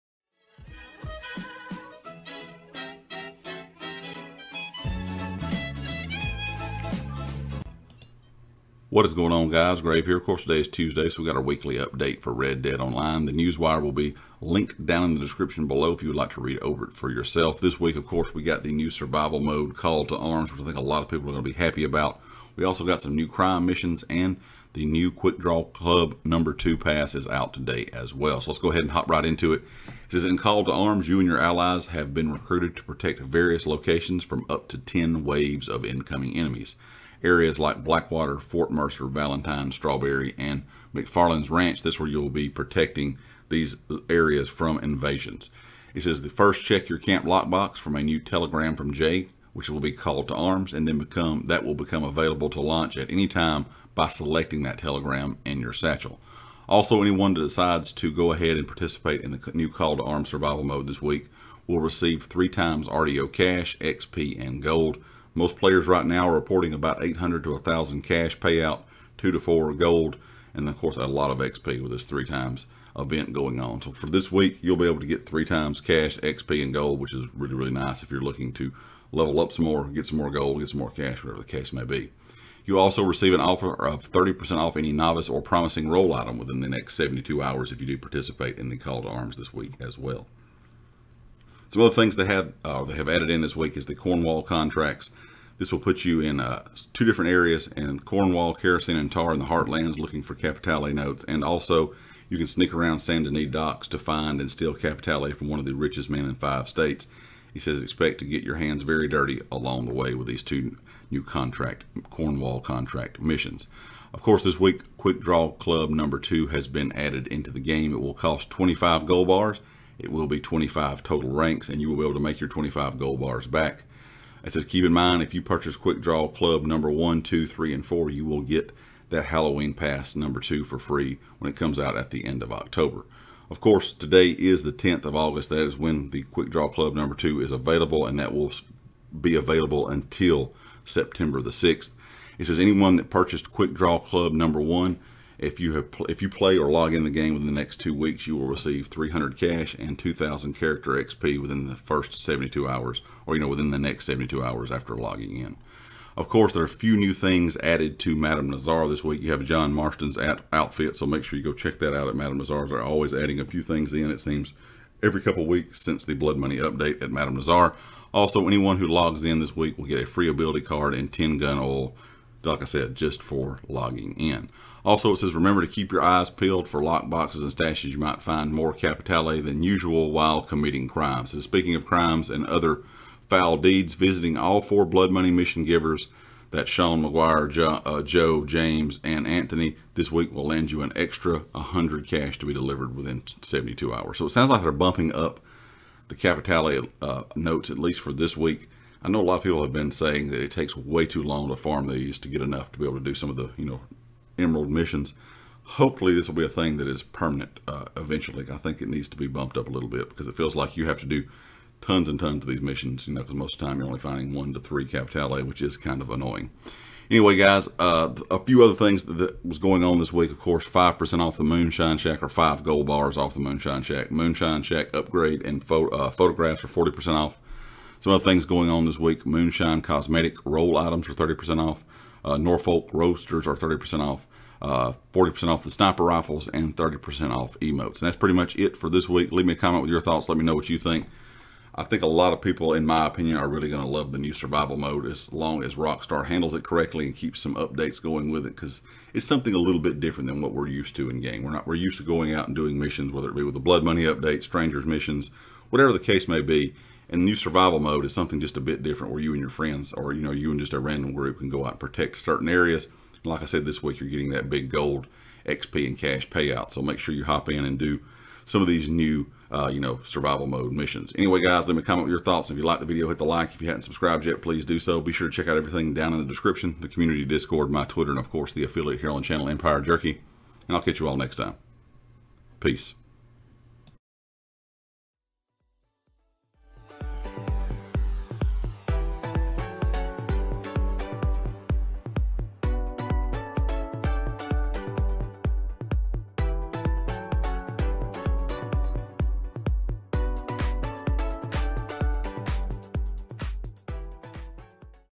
Red Dead Online Weekly Update Aug.10th | New Survival Mode Update
This weeks red dead online update brings the new survival mode, quick draw pass 2 and some new crime missions.